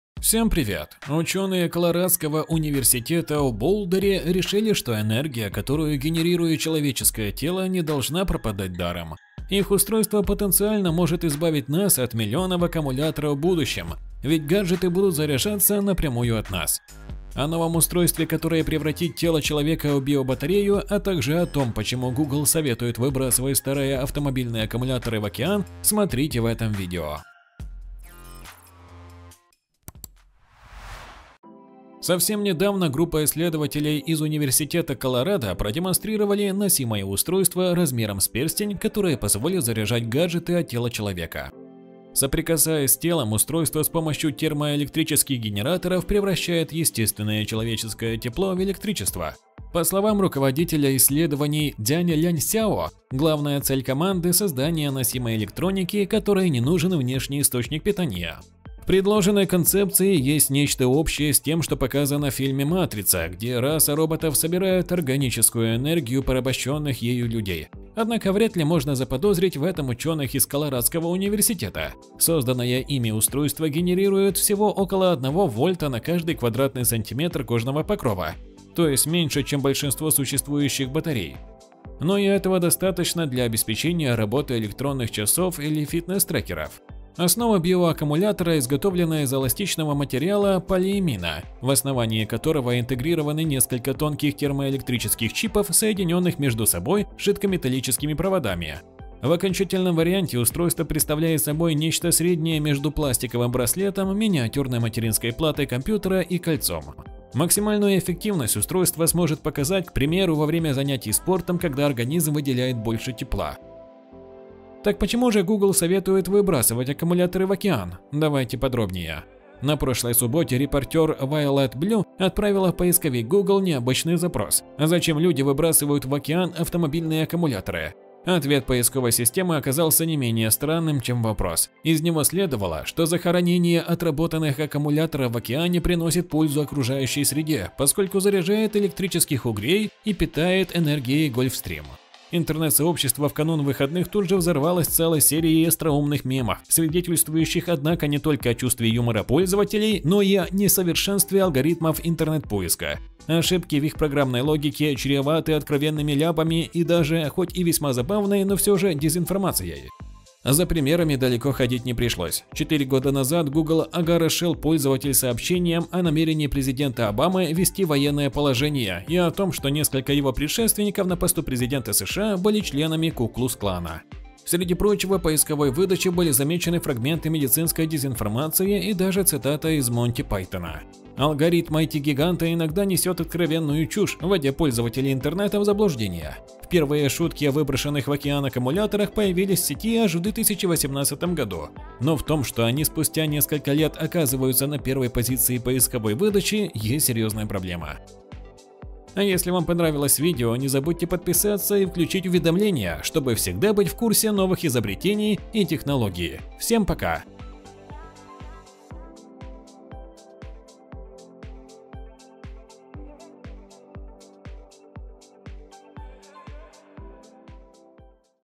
Это кольцо превратит ваше тело в аккумулятор | Новые технологии 2021
Устройство которое превратит ваше тело в аккумулятор. И почему Google советует выбрасывать аккумуляторы в океан?

Ученые Колорадского университета в Боулдере решили что энергия, которую генерирует человеческое тело, не должна пропадать даром. Их устройство потенциально может избавить нас от миллионов аккумуляторов в будущем, ведь гаджеты будут заряжаться напрямую от нас. О новом устройстве которое превратит тело человека в биобатарею а также о том, почему Google советует выбрасывать старые автомобильные аккумуляторы в океан - смотрите в этом видео.

RoboTube - Новые технологии 2021, наука и техника, новости технологий, изобретения.